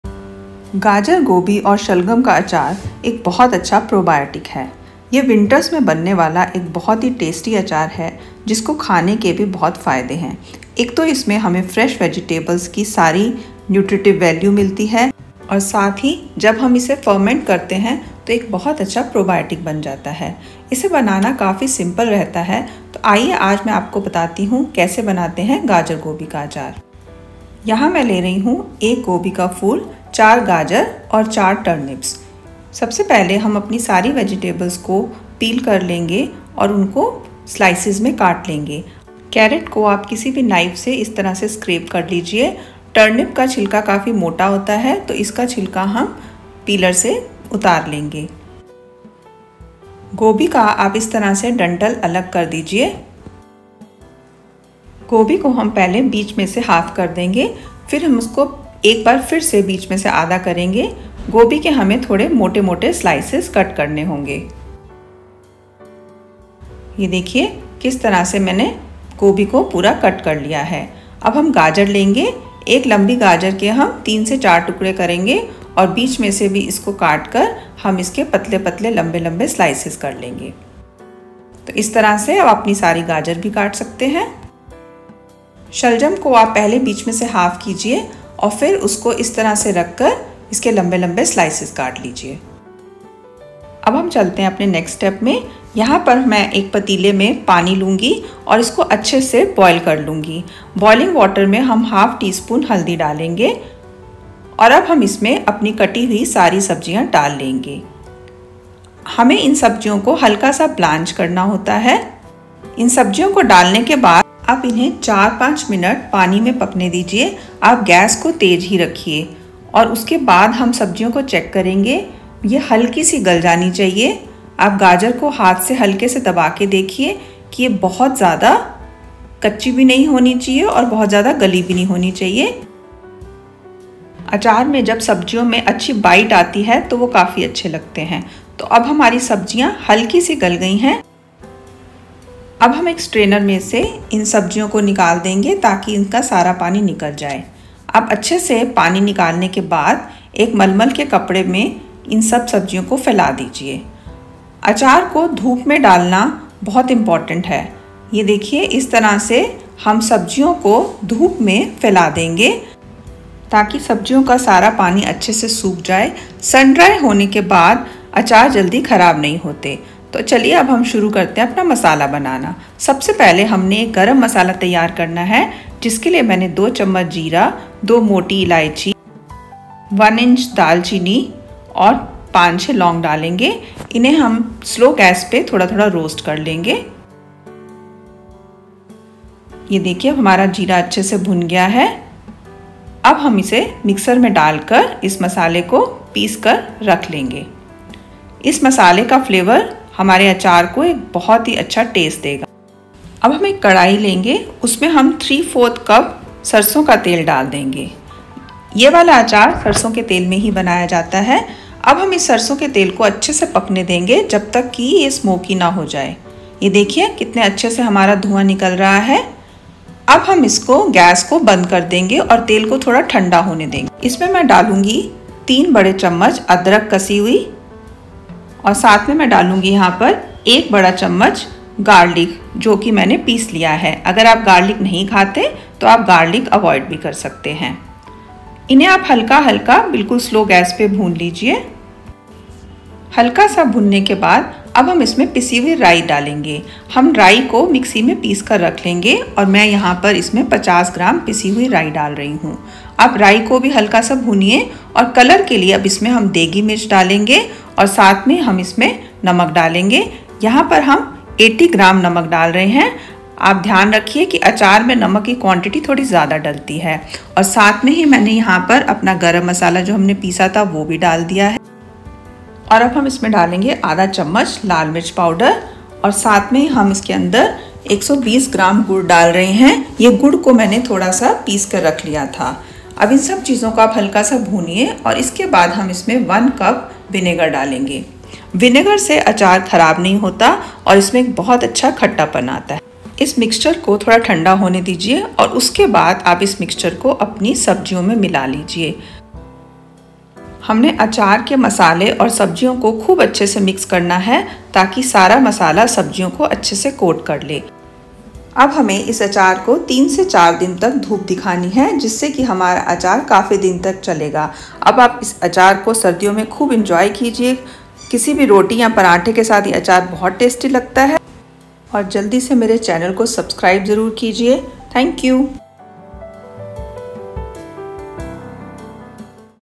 Gajar Gobhi Shalgam Achar in Hindi | Winter Season Special Pickle
Gajar, Gobhi and Shalgam Achaar Recipe | Very Healthy Winter Pickle | Winter Special Recipes | गाजर गोभी शलगम अचार कैसे बनाए | Vegetable Pickle

Ingredients:
1 kg - Gajar, Gobhi, Shalgam (After peeling and cutting)
Water to boil
1/2 tsp haldi

For Garam Masala:
2 Tbsp Cumin Seeds
1 inch Cinnamon Stick
2 Black Cardamoms
5-6 Cloves

3/4 Cup Mustard Oil
3 Tbsp Grated Ginger
1 Tbsp Crushed Garlic (Optional)
50 gms Rai - Grinded
80 gm Salt
2 tsp Kashmiri Red Chilli Powder
1/2 tsp Red Chilli Powder
120 gms Crushed Jaggery
1 Cup White Vinegar

Notes:
1. Sun dry the vegetables after boiling for 4-5 hours.
2. Keep the pickel in sun for 3-4 days so that the it lasts longer and all flavours are properly absorbed.
3. Adjust red chilli powder according to your preferences.

Hope you enjoy the recipe!